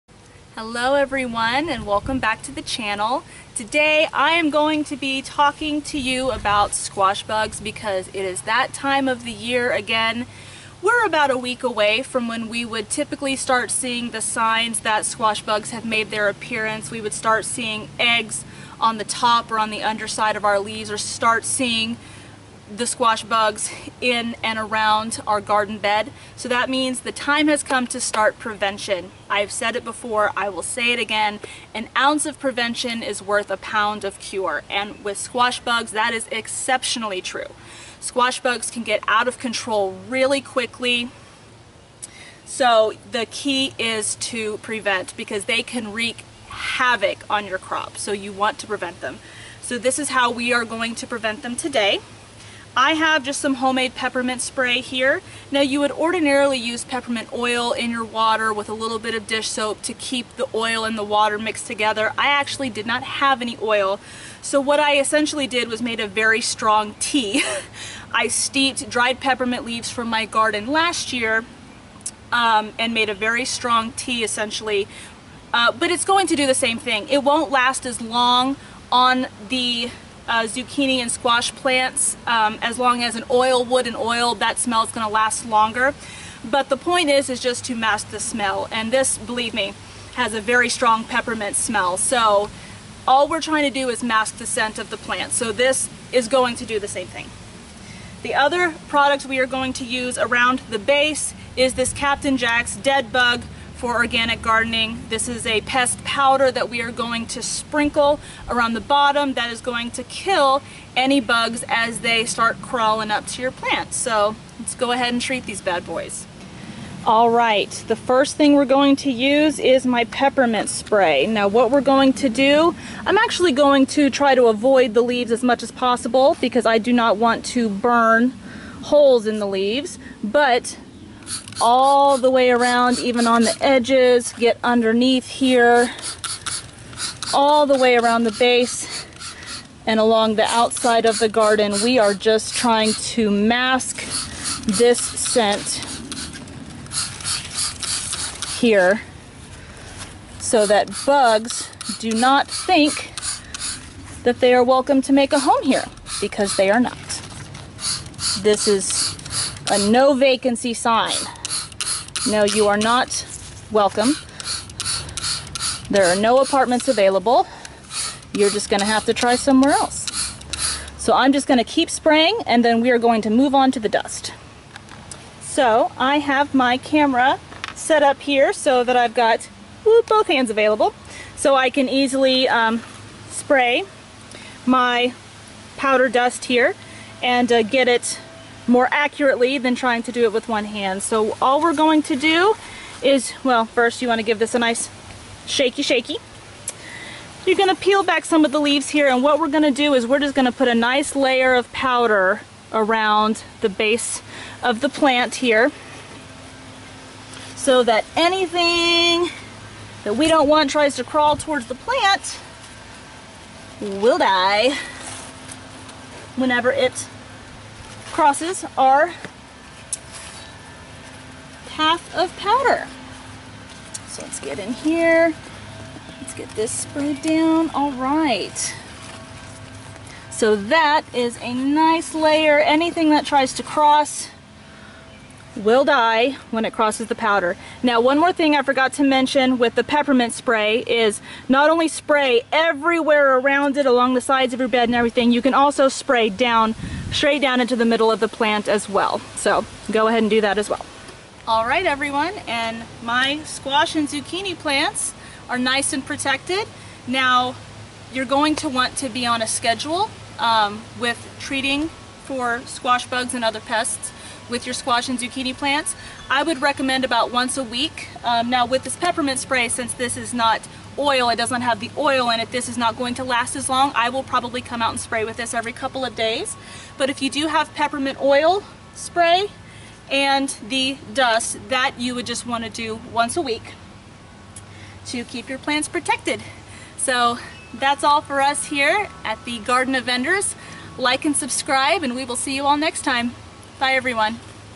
It's Almost Squash Bug Season!
Prevent those NASTY squash bugs from wreaking havoc on your crop with these helpful tips! Enjoy!